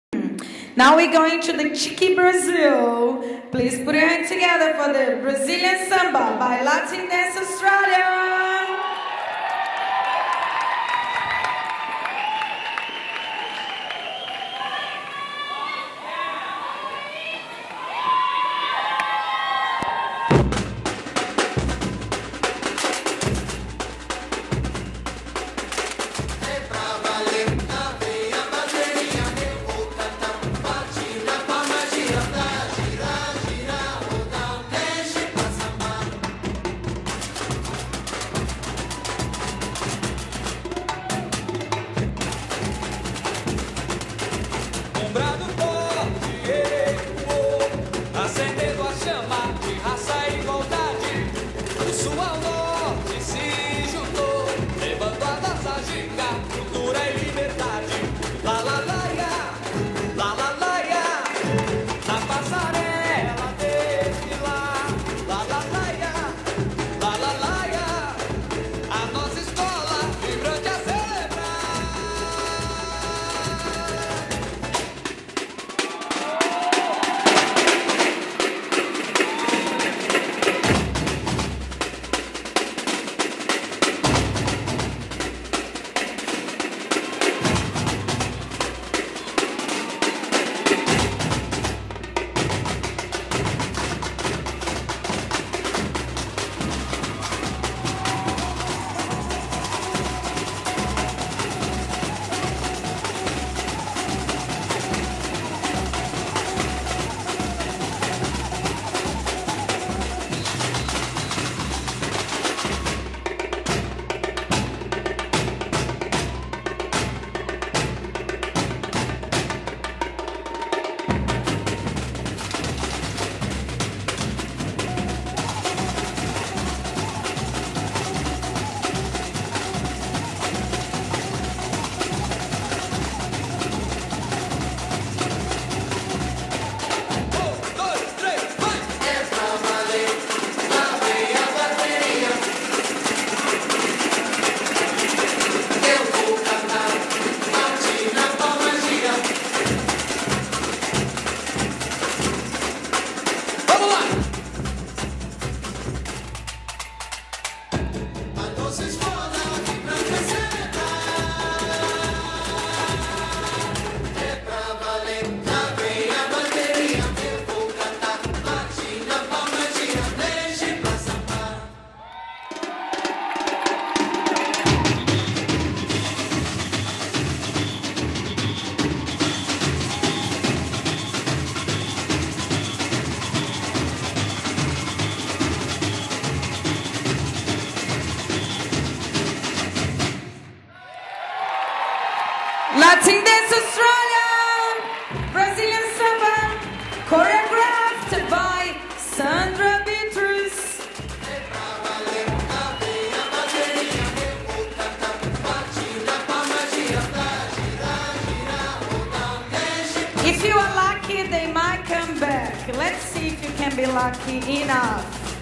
Australian Dance Festival 09 Samba by Latin Dance Australia
Brazilian Samba very Cheeky Choreographed by Sandra Betrus from Latin Dance Australia.

Produced and directed by Angela Lau & Marcia Pinheiro Percival